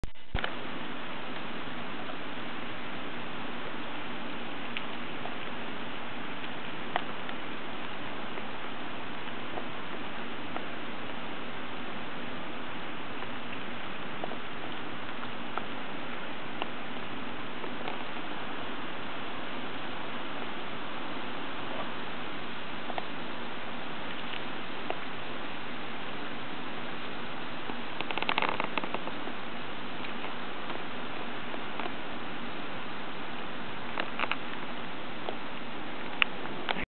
Cissy eating grapes
These are wild raccoons.

Cissy is eating grapes I tossed her. Dennis seen exiting over the fence. With him gone, Cissy comes over to see what I have.